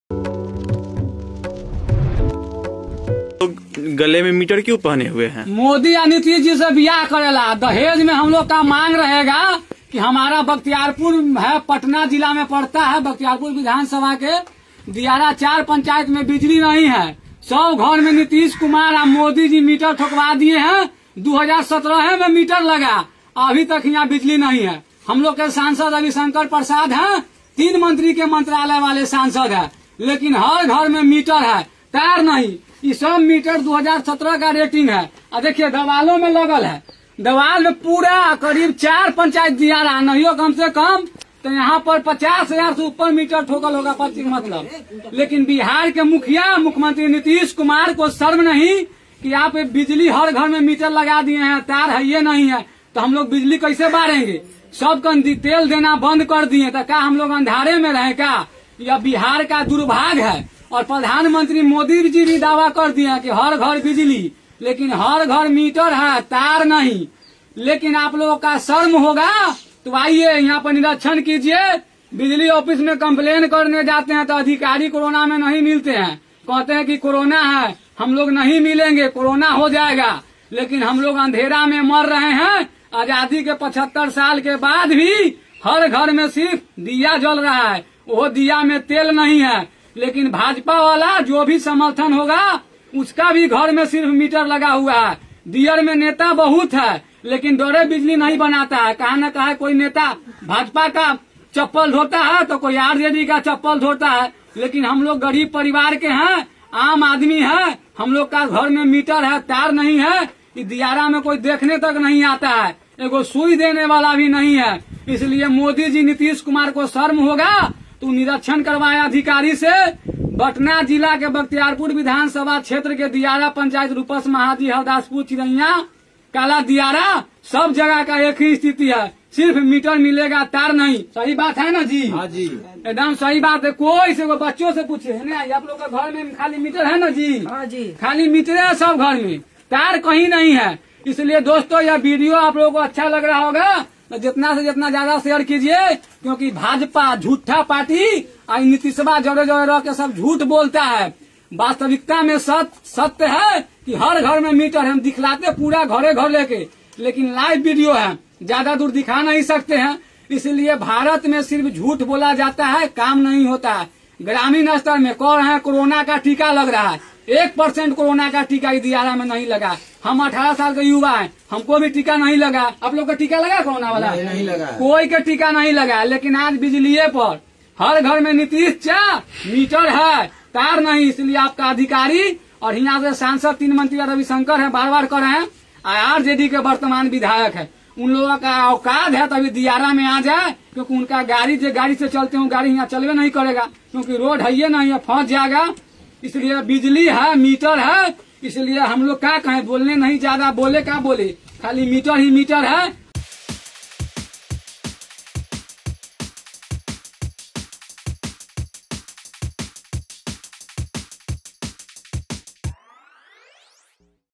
PM Modi और Nitish से शादी करने निकला ये लड़का, मांग लिया दहेज़ में ये सब जानिए क्यों... ! TheHDNews
Anchor: